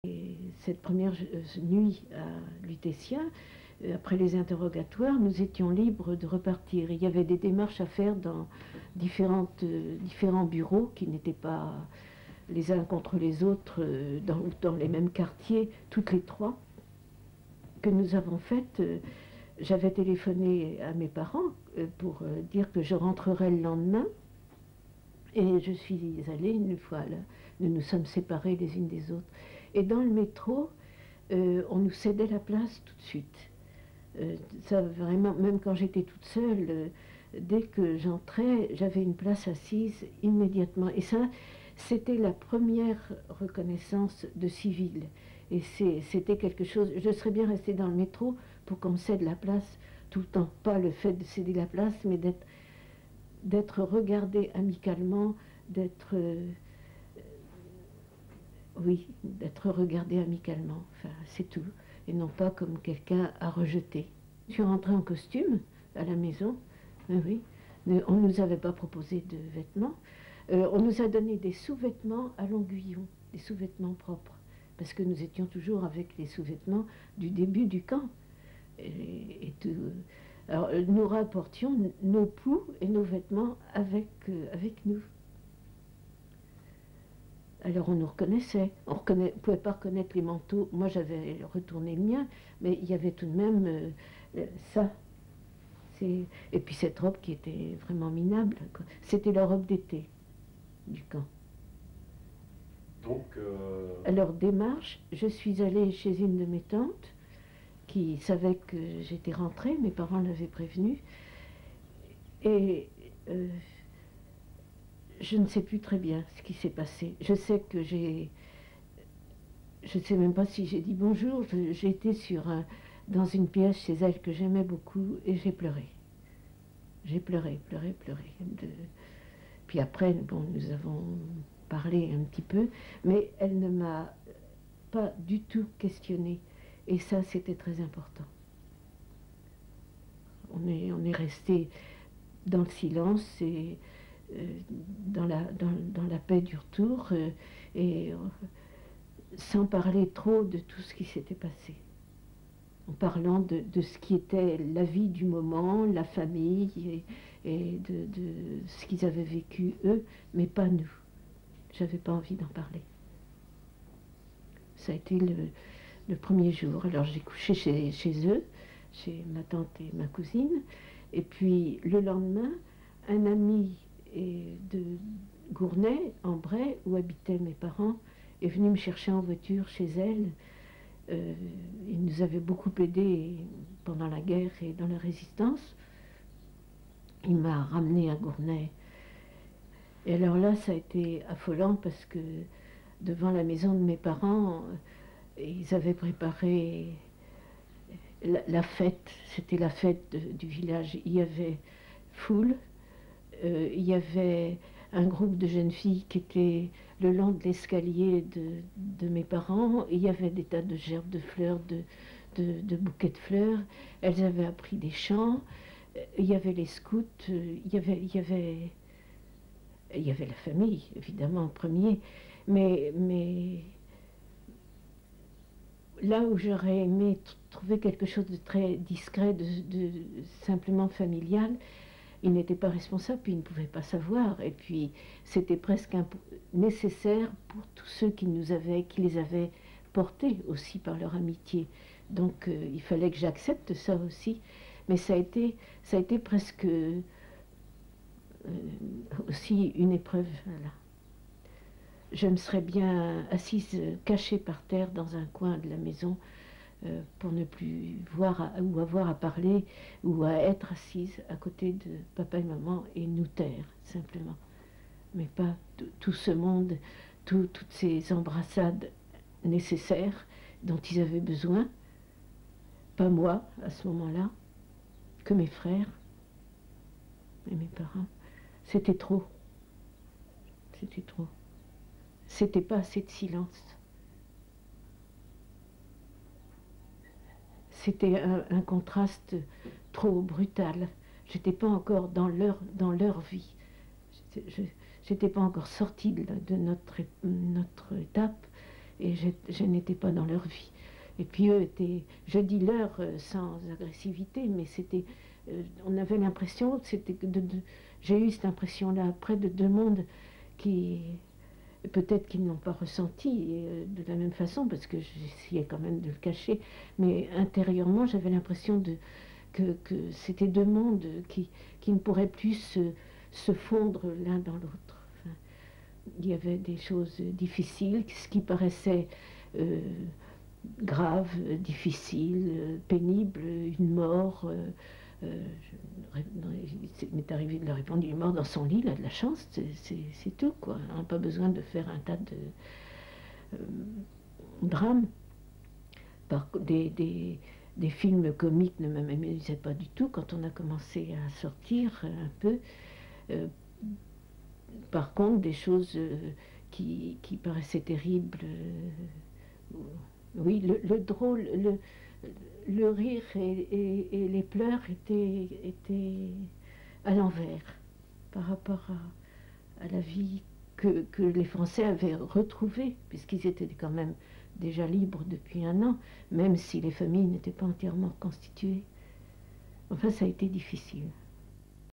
Témoignage Thérèse Verschueren | CNRD 2014-2015
Thérèse Verschueren est née en 1918. Elle est arrêtée le 23 septembre 1943 à Paris en raison de ses actions dans la résistance et est déportée à Ravensbrück et à Zwodau. Elle s'évade en compagnie de deux autres déportées en mai 1945 lors de l'évacuation à pied du camp de Zwodau vers Dachau. Dans cet extrait, elle évoque son retour à Paris et l'accueil de sa famille.